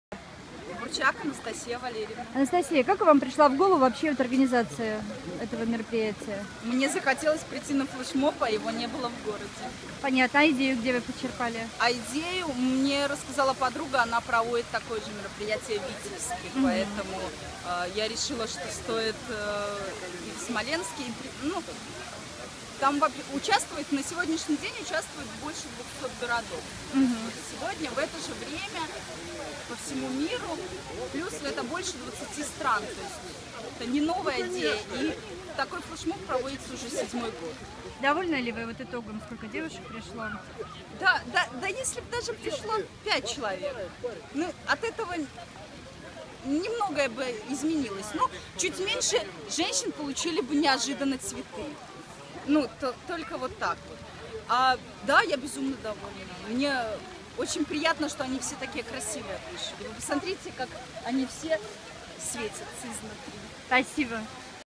Флешмоб женственности в Смоленске
В Смоленске впервые прошёл Международный флешмоб женственности